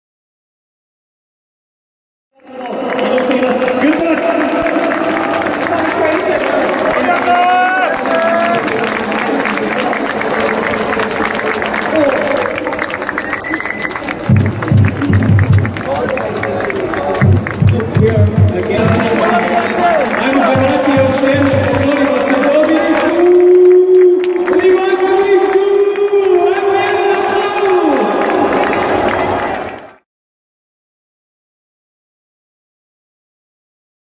Hajime Moriyasu🇯🇵 Japan bowing to fans in gratitude What can he teach LEADERS
What Did Japan football Coach Hajime Moriyasu Do after Japan Lost, what can we learn from his act, how do those country men have such a humility WOW, Coming  out in front of their country men and bowing in gratitude, what i had experienced during that match i will remember for life, people of japan i met during that match to coming experience and cheer for their country and accept their loss as if it was their own .. they fought like warriors  in all their games and won millions of hearts across the globe.

Big salute to Japan and incredible team and positive attitude they had shown in that big stage.
Madhu